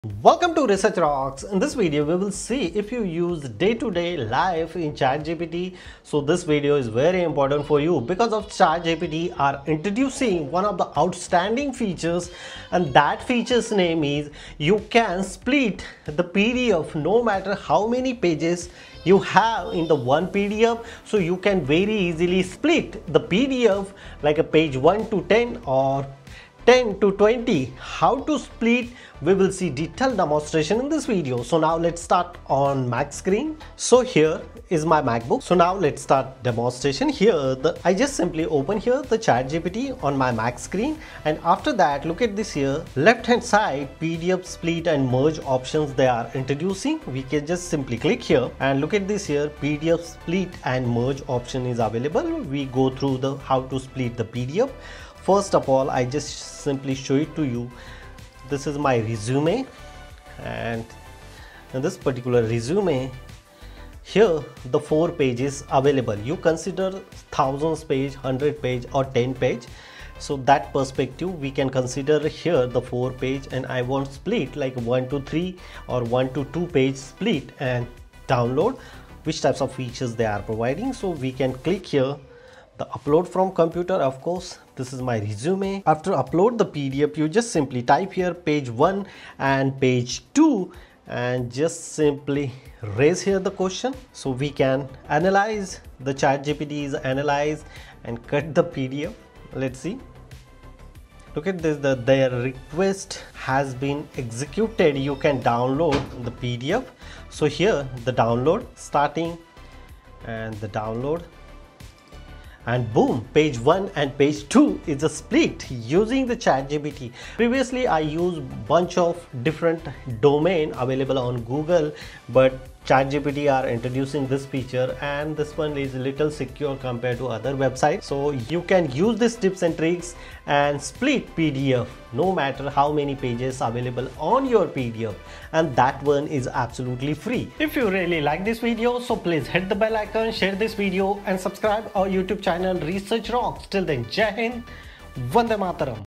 How to Split PDF using ChatGPT for Free
Discover how to split PDFs quickly and effortlessly with ChatGPT—for free! In this step-by-step guide, learn how to break down large PDF files into smaller sections using AI. No additional software or complicated processes required! Perfect for students, professionals, and anyone managing PDFs. Watch now to simplify your workflow and boost productivity.